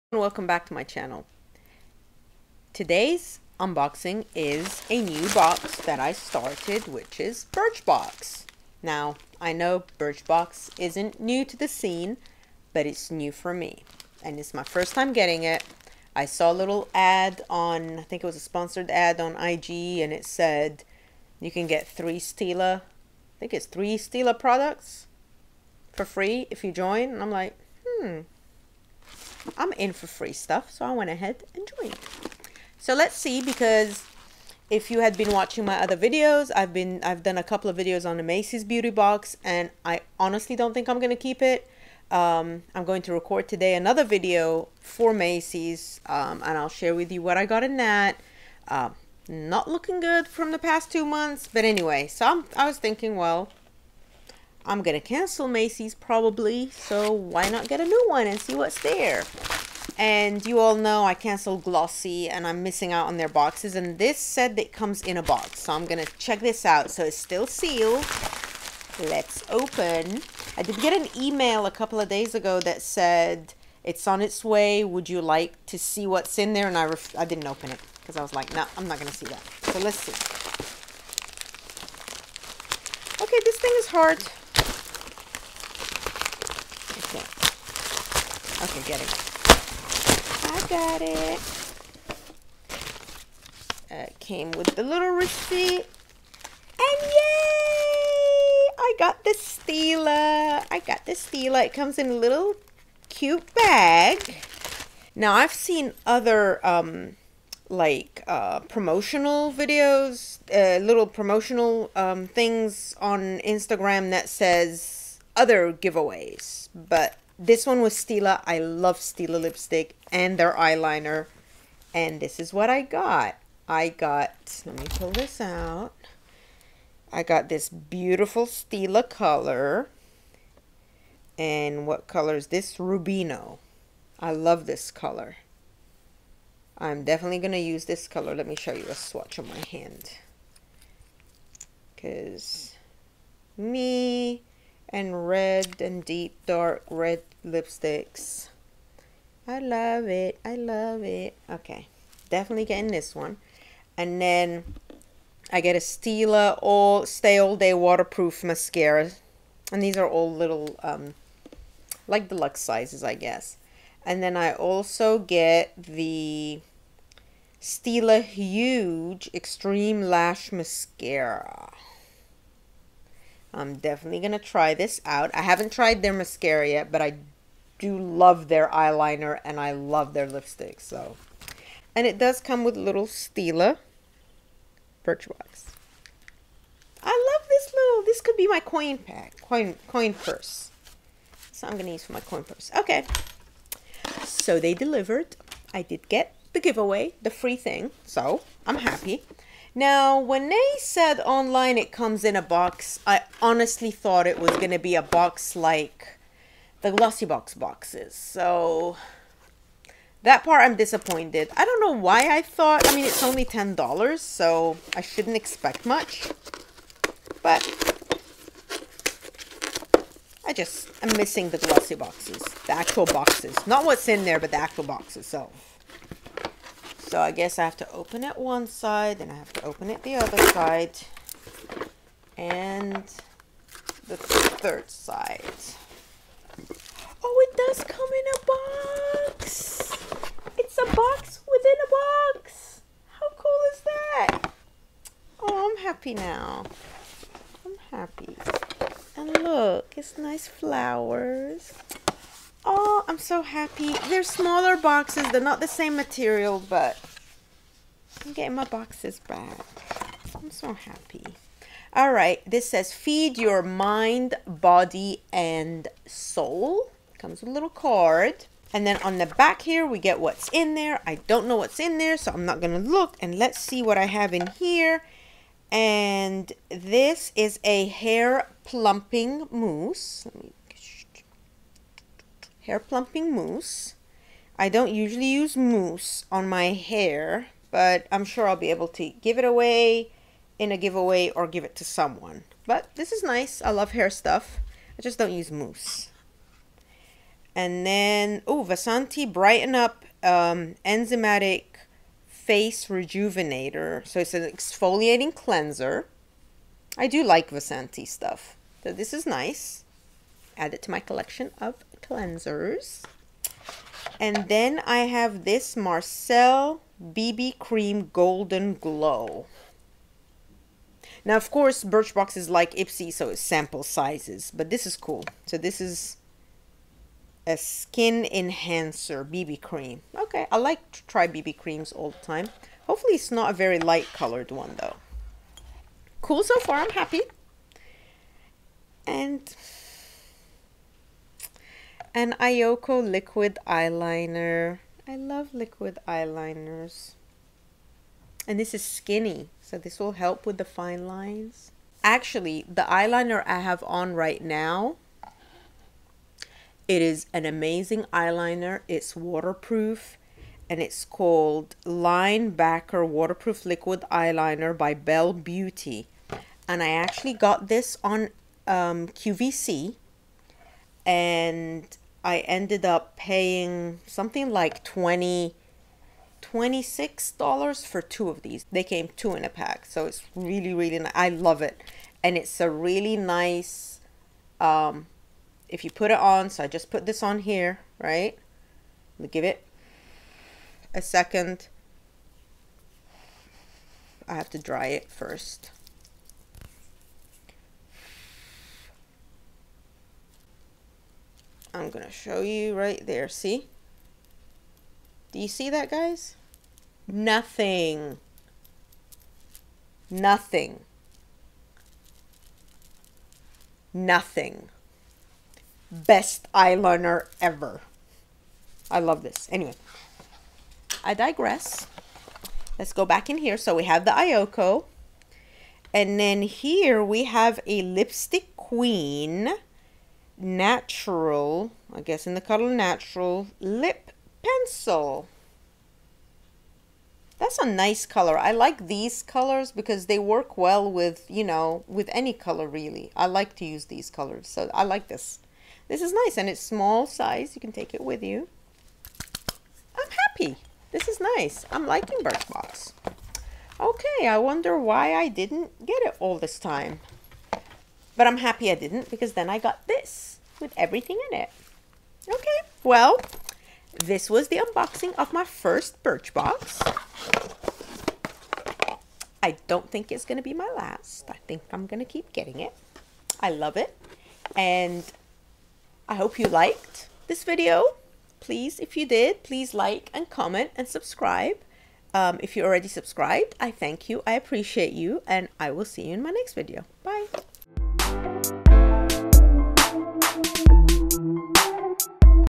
My first Birchbox!!
My first Birchbox!! Watch to see what I got and what I think about it.

Eyeliner product that I showed from QVC:
Belle Beauty by Kim Gravel Line Backer Liquid Eyeliner

Are you a Business/Company?

Prpducts in box:
~ Eyeko Skinny Liquid Eyeliner
~ Vasanti® BrightenUp!® Enzymatic Face Rejuvenator
~ Marcelle BB Cream Golden Glow
~ Oribe Grandiose Hair Plumping Mousse
~ Lipstick Queen Lip Liner